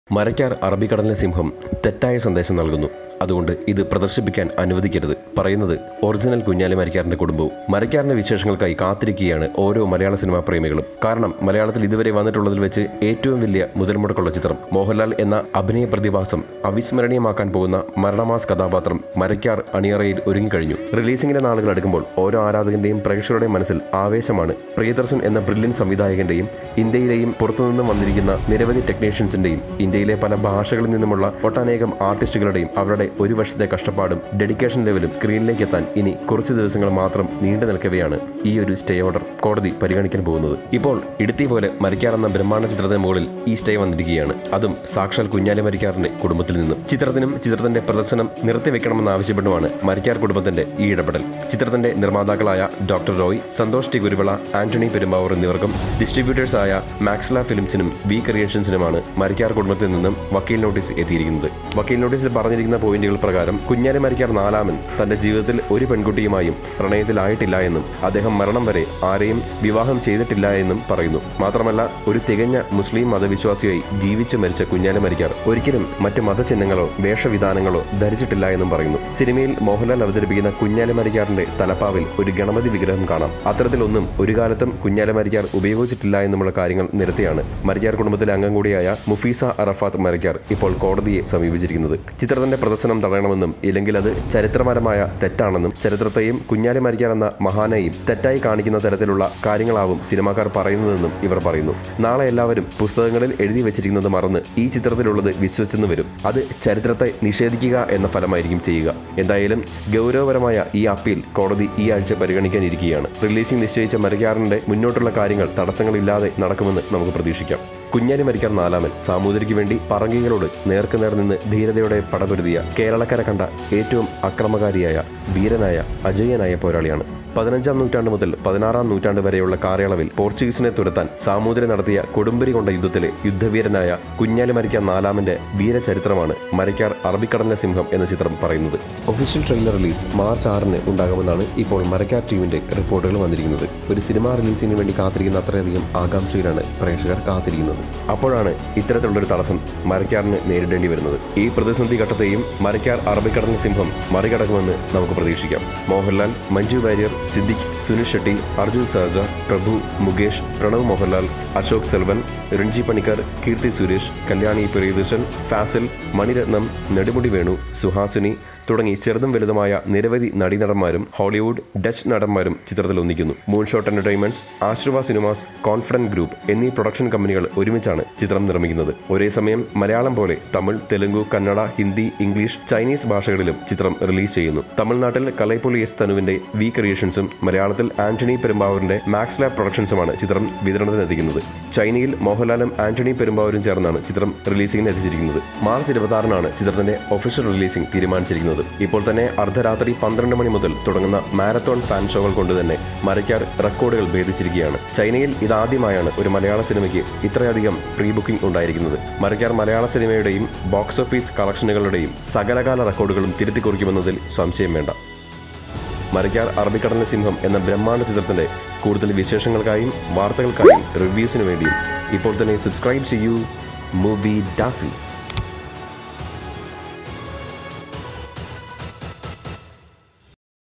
Mohanlal Starrer Kunjali Marakkar Biopic Runs Into Controversy | Hot Cinema News
A descendant of Kunjali Marakkar has filed a writ petition in the Kerala High Court seeking to stop the release of the upcoming Mohanlal-starrer Arabikadalinte Simham (Marakkar: Lion of the Arabian Sea) based on Kunjali Marakkar IV, the naval chieftain of the Zamorin of Calicut, “for distorting the history” of the 16th century hero.

The suit, filed by Mufeeda Arafath Marakkar, wife of Muhammed Yaser Arafath Marakkar of Koyilandy, says the big-budget moview with mega stars “disseminates a wrong message to scholars and researchers” through a perverted version of the history of the Marakkars.

The epic historical drama, said to be the most expensive Malayalam film ever made, directed by Priyadarshan is scheduled for release on March 26.